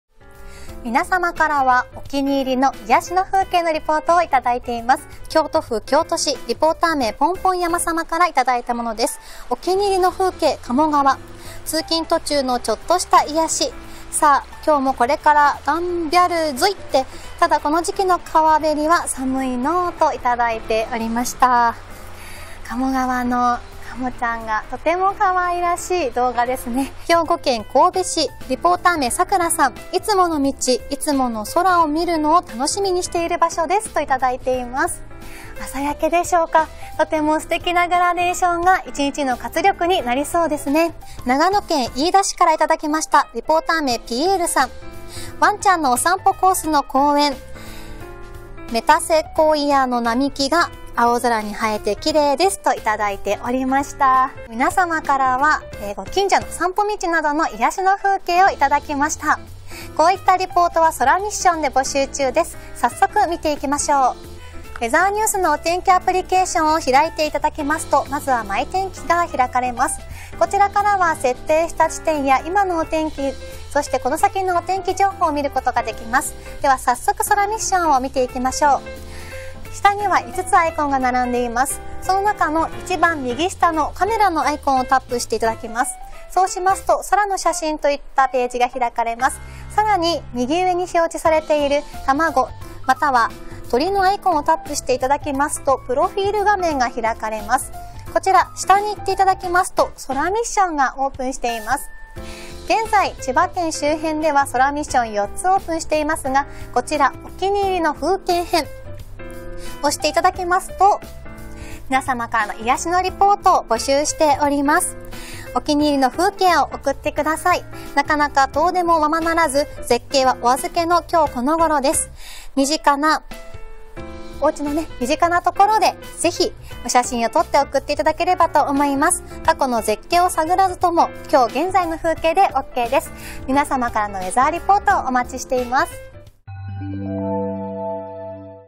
癒やしのリポート「お気に入りの風景」
空で繋がる仲間と、ホッとする空や季節の風景を共有して、ステイホームが少しでも楽しく過ごせるよう、ウェザーニュースでは週替わりで癒やしのリポートを募集中。今週は「お気に入りの風景」を募集しています。みなさんからは素敵な映像が届いています。